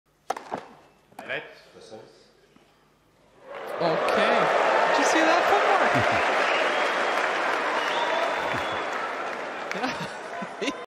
Rafael Nadal with impressive football skills during Wimbledon
Rafael Nadal impresses watching England manager Gareth Southgate with some close control.